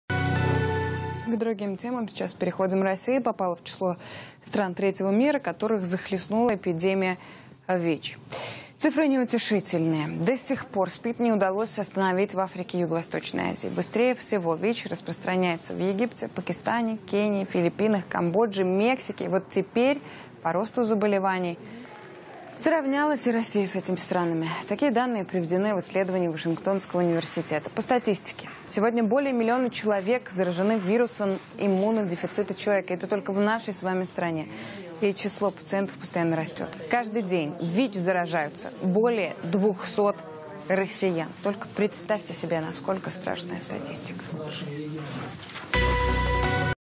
Россия попала в группу стран третьего мира по масштабу распространения ВИЧ
Число заболевших ВИЧ в России постоянно растет